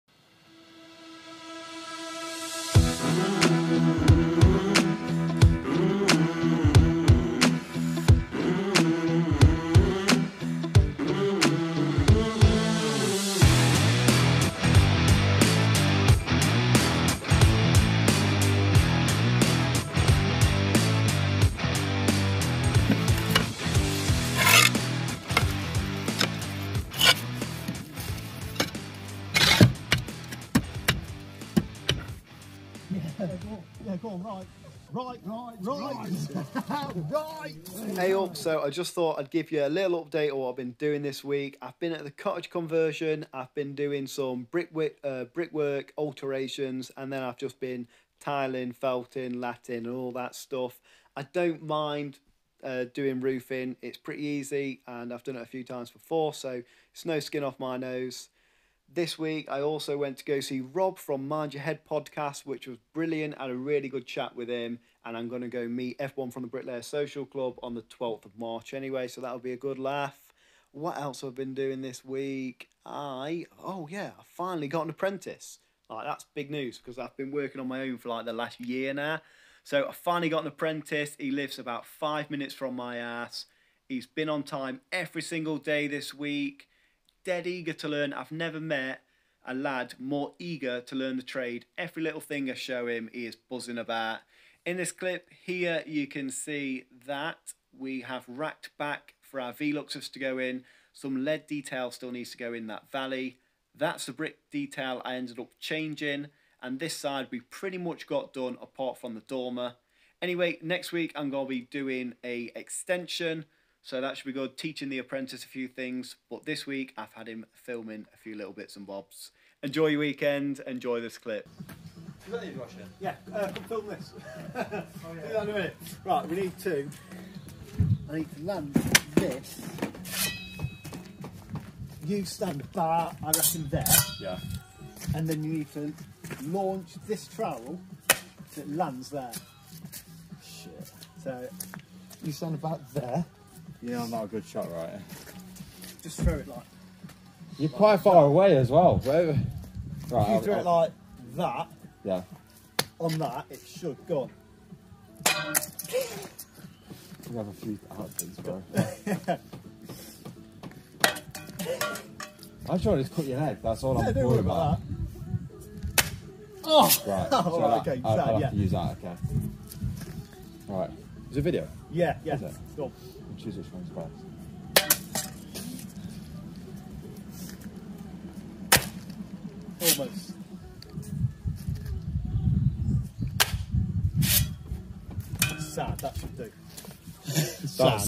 Izzy The Roofer? 😂 NEW APPRENTICE🙌
Updated on the cottage Extension and bit of talk about the new apprentice and what's coming up next week